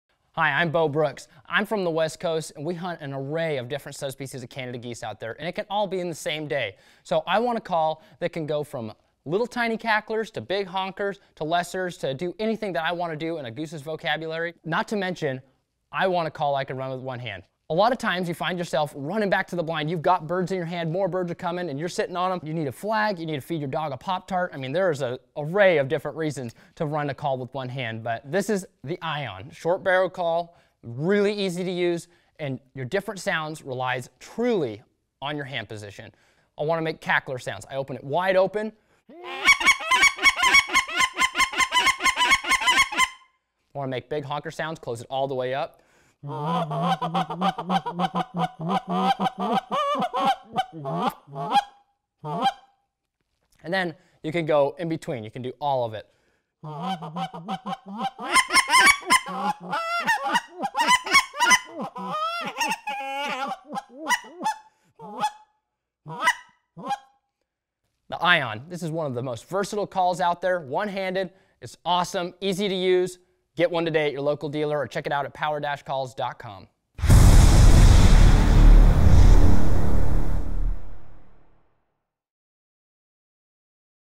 Power Calls - ION
The Ion is simply built for speed and versatility.  Designed with one-handed operation in mind, the short body of the call allows for maximum variability in calling styles, without the need for added hand back-pressure.  This is crucial when operating a flag. The Ion comes with our molded broken-in Lightning guts that provide a realistic tone from top to bottom.

Product DNA
-Structure: Polycarbonate/Acrylic Blend
-Coloration: Battleship Gray
-Core Guts: Polystyrene Worn-In Toneboard
-Reed: Mylar PreciseFit Reed
-Packaging: Clamshell